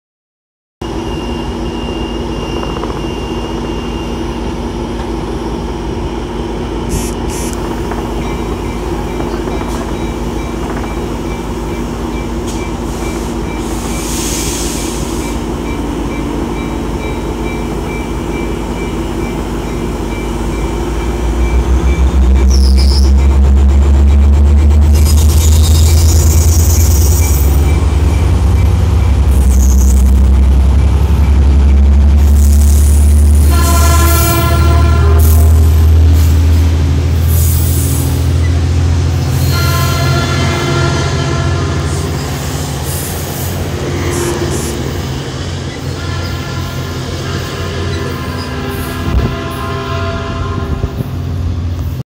865 leads train out of Fontana station
Cabcar 604 is in the consist. As well as Cabcar 639